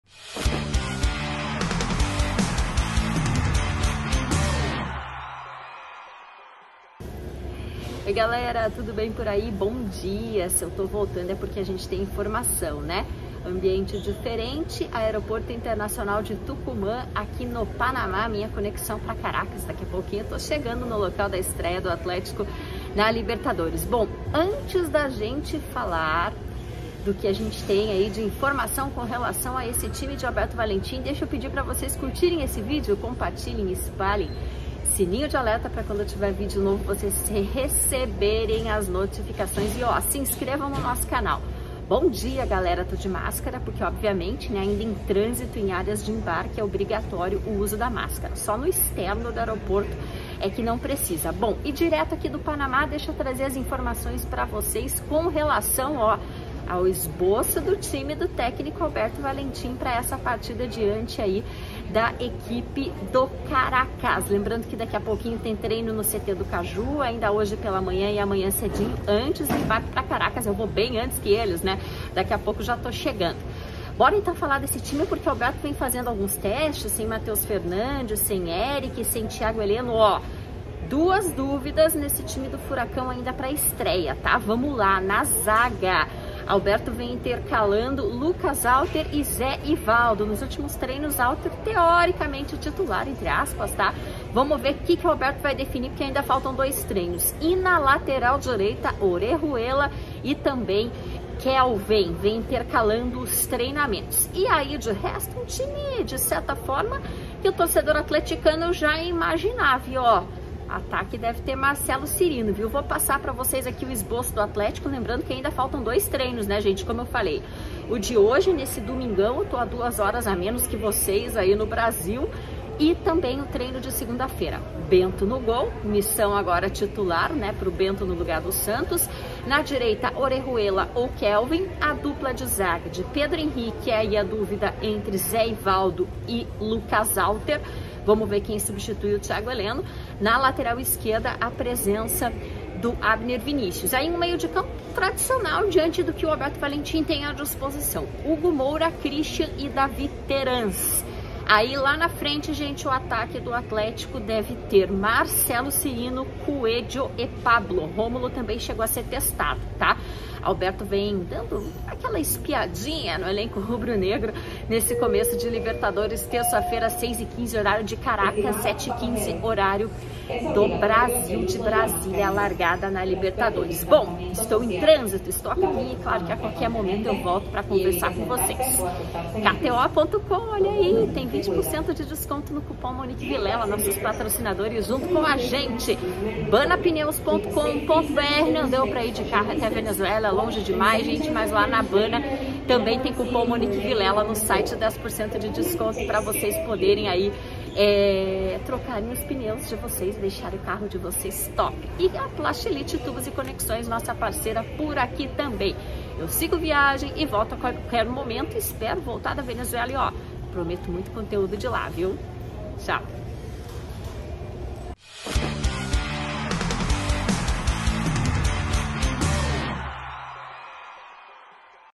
ALBERTO VALENTIM ESBOÇA ATHLETICO PRA ESTREIA NA LIBERTADORES; DÚVIDAS NA ZAGA E LATERAL- 03/04/2022
Furacão larga na competição contra o Caracas, na Venezuela; time rubro-negro ainda tem dois treinamentos antes do primeiro jogo.